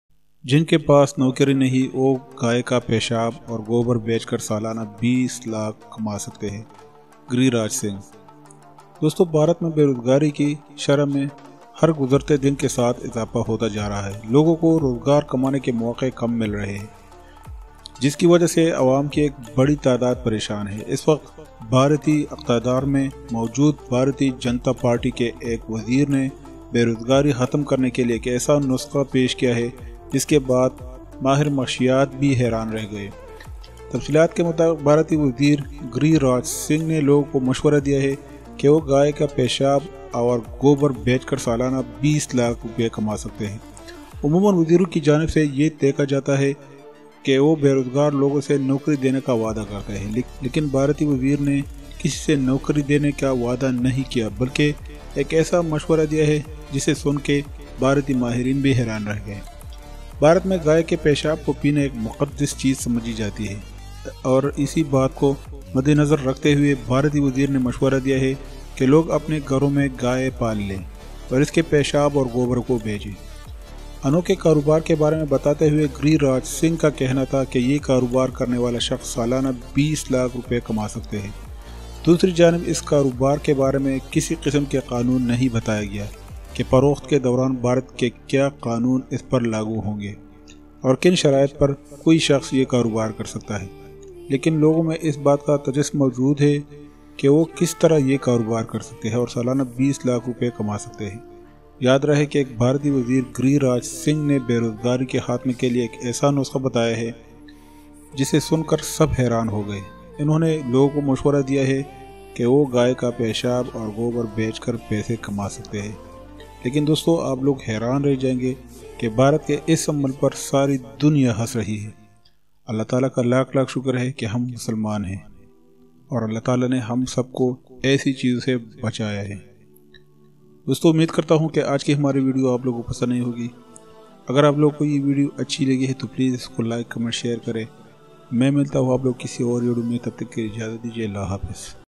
The stupid modi indian planing to seling Cow Urine and Dung
In India, drinking cow's urine is considered a sacred thing. In view of this, the Indian Minister has advised that people should raise cows in their homes and sell their urine and dung. Gary Raj Singh says that the business person can earn 2 million annually. On the other hand, there is no law of the business regarding what the laws of India will apply to it during the sale and on what conditions a person can do the business.
The unemployment rate in India is increasing with each passing day. People are getting less chances of earning jobs, which is causing a huge number of people to worry. The minister has presented a prescription for ending unemployment after which economists are also surprised.